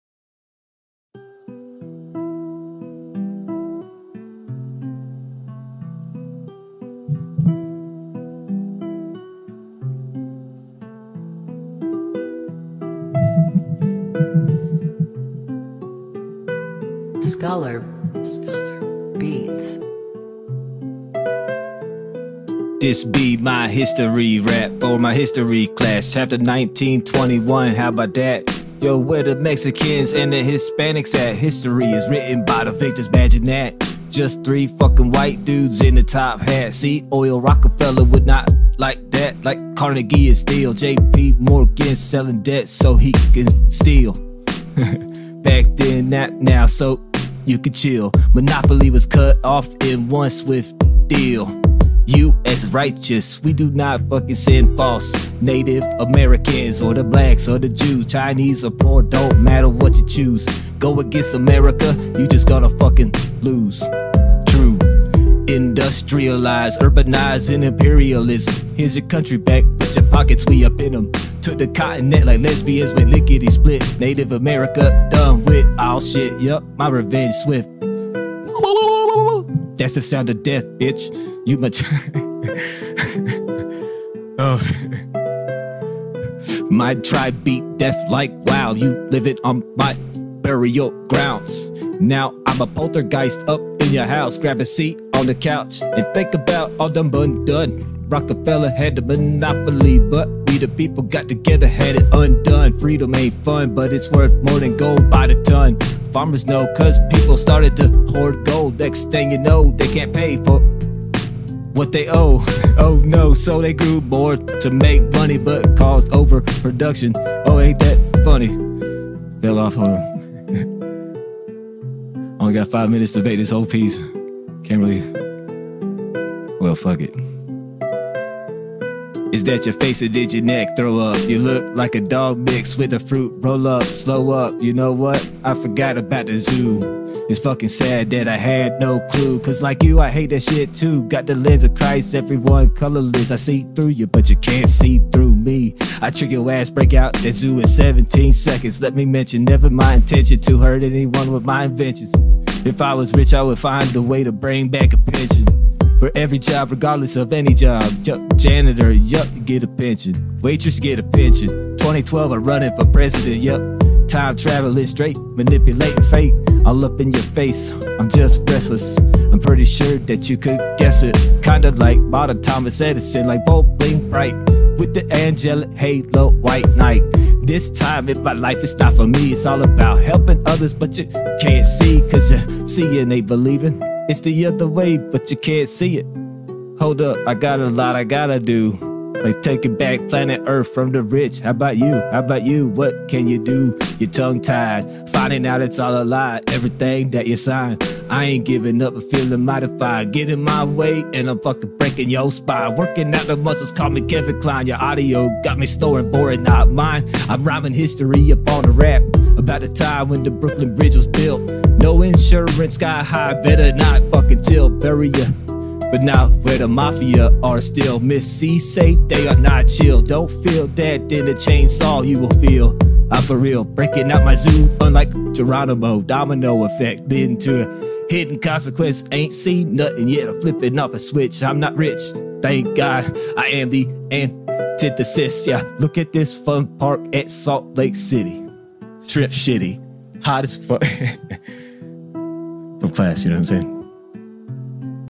HistoryRap
Did in one take for a class I thought I had another day to make, I had a bunch of jumbled down shit mixed in with a little bit of written topics I went over from memory as I walked 12 miles to an from school during the day.  First try at anything on the beat, not organized and I just randomly time-shifted it and got it right.  I did pray too ten minutes before it was due that time was running out to start :O